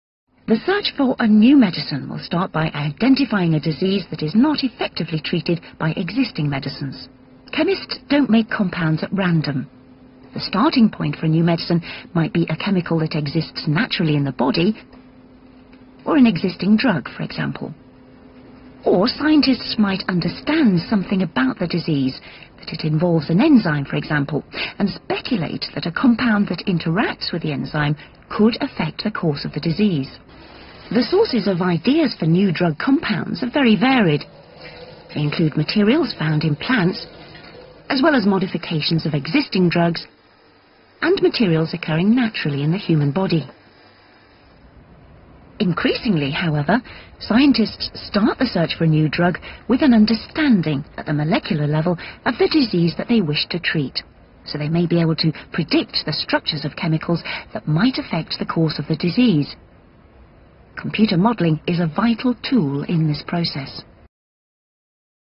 The Search Begins
A PowerPoint presentation that contains several video clips to illustrate the process of drug discovery and development, including target identification, drug discovery, lead optimisation, pre-clinical testing and clinical trials.